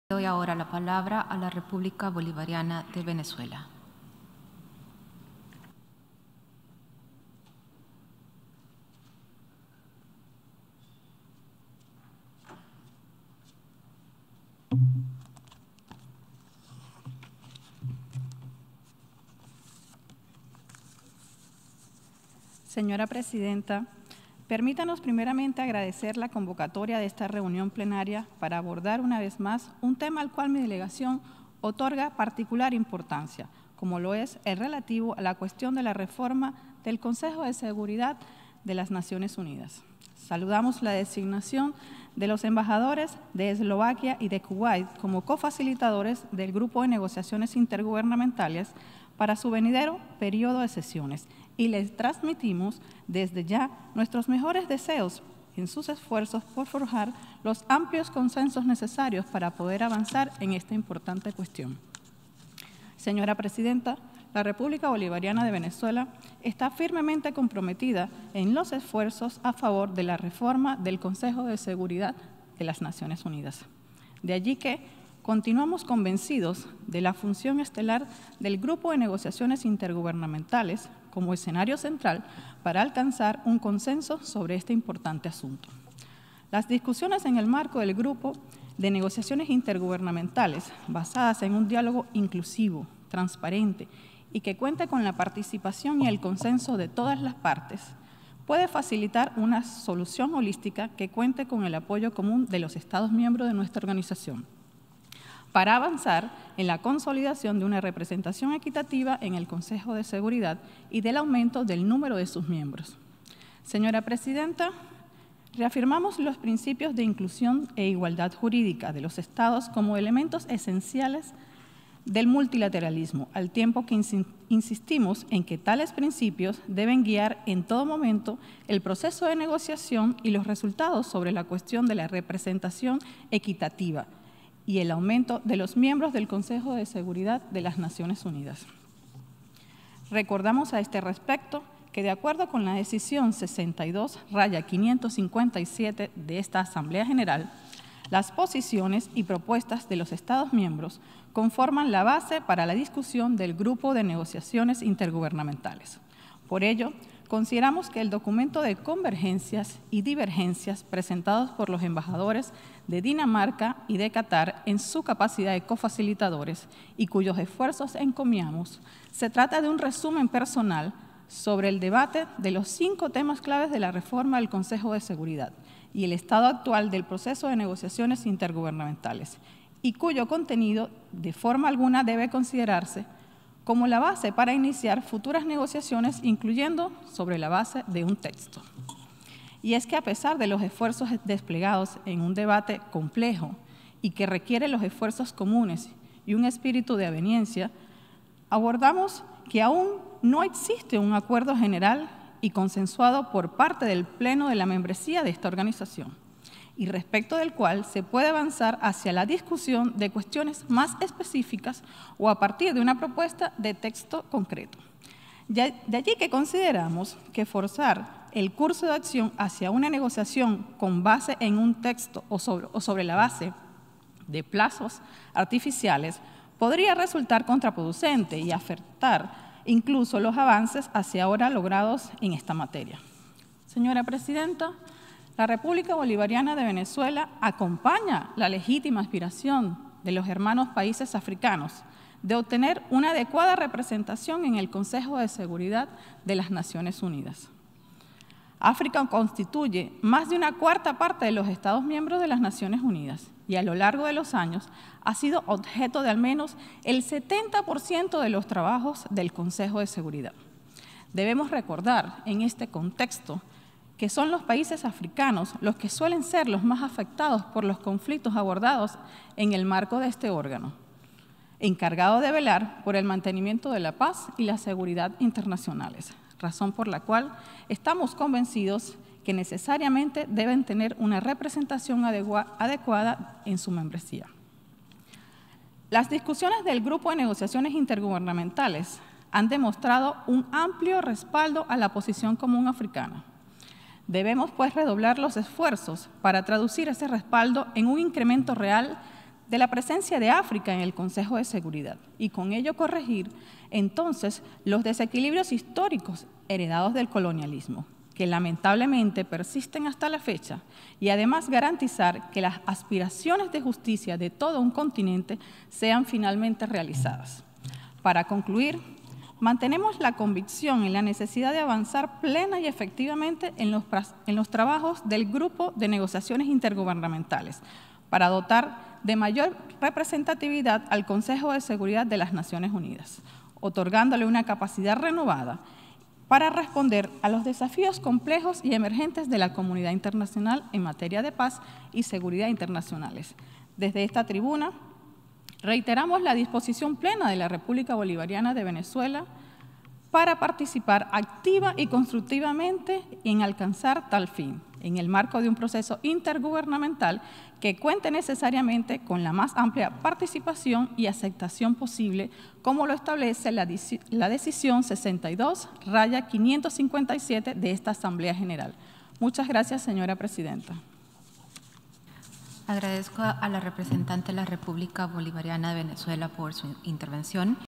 Venezuela - Debate sobre representación equitativa en el Consejo de Seguridad
Venezuela participa en el Debate de la Asamblea General sobre la “Cuestión de Representación Equitativa en el Consejo de Seguridad y el Aumento del Número de sus Miembros y otros Asuntos relativos al Consejo de Seguridad” reafirmando el principio de igualdad soberana de los Estados.